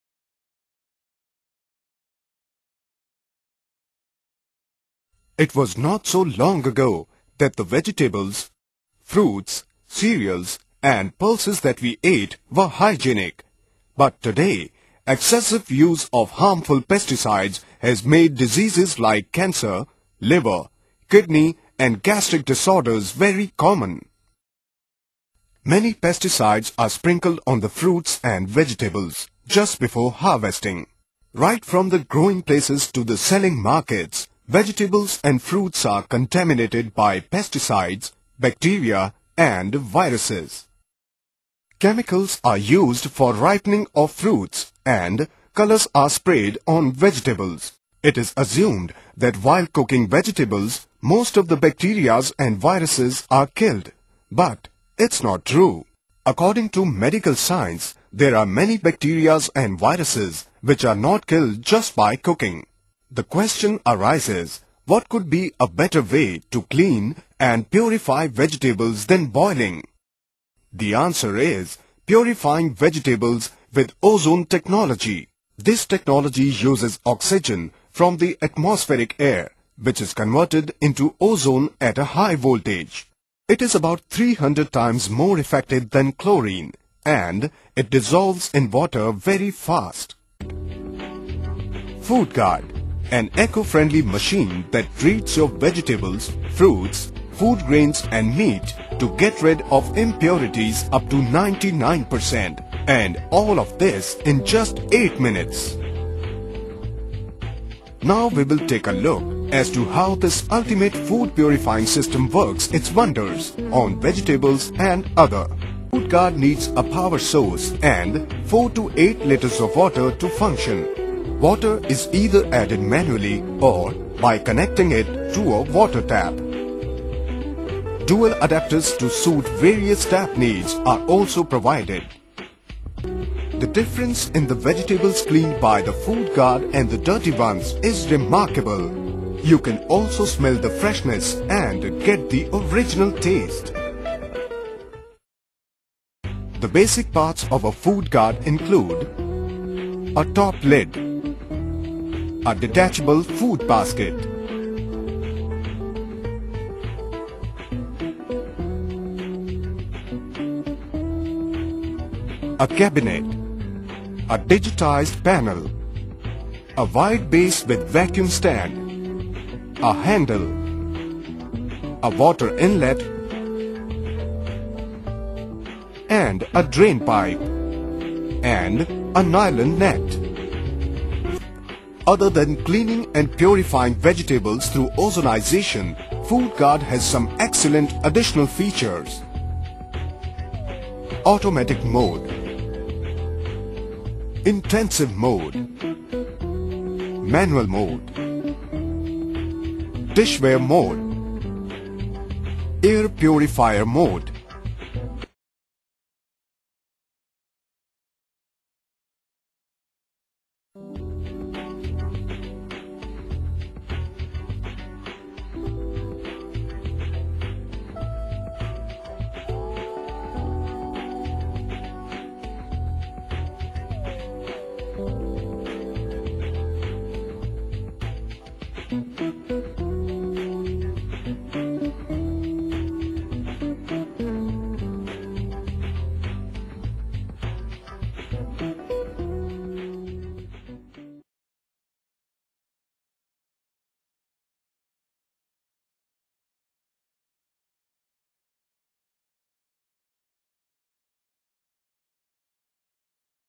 Ozone Fruits Vegetable Food Purifier Washer to clean pesticides and chemicals - Food Guard
- Removes pesticides,bacteria and viruses from vegetables, fruits and foodgrains.
- Ensures safest purest food possible
- Utilizes OZONE technology for purification
- Increases shell-life of fruits and vegetables
- Reduces fat from chicken (for people who are non-vegetarian)
- Completely chemical Free
- Fully Automatic
- Sterilizes and disinfects dishware
- Works as Air Purifier
- Environment Friendly
- Washes even the softest vegetables and fruits
- Low Maintenance cost
- Certified in India by CLL (Choksi Laboratories Ltd)
- Approved by EIL in US, CE in Europe, GS in germany